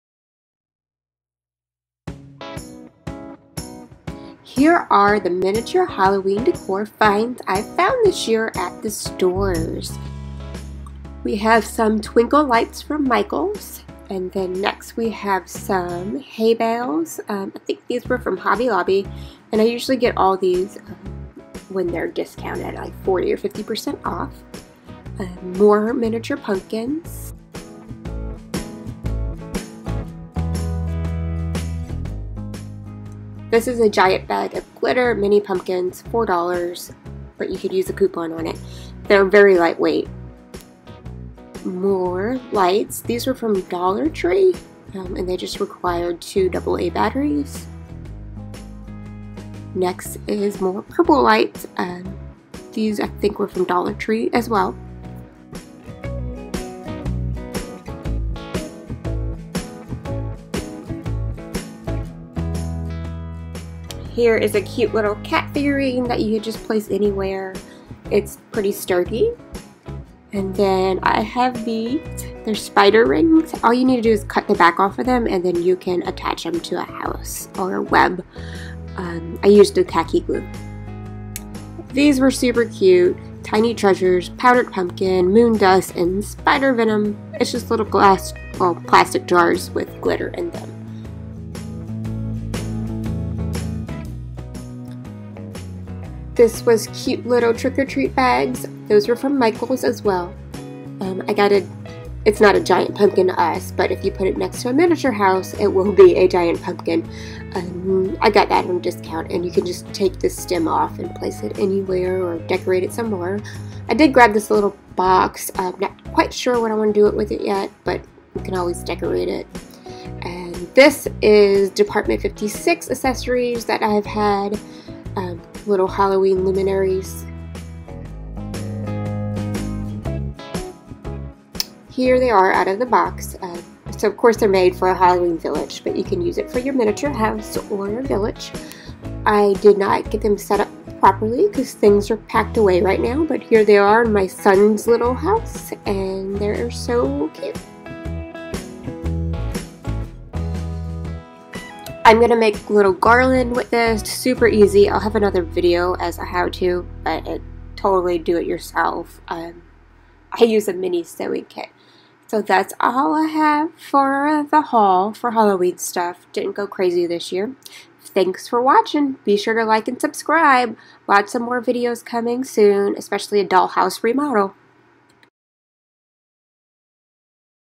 Miniature House Decor | FALL & HALLOWEEN Haul for Decorating | Miniature Hobbyist
Today's video is a quick video of my finds for Halloween/fall decor for miniature houses/dollhouses/villages.   Includes items from: Michael's, Dollar Tree, and Hobby Lobby.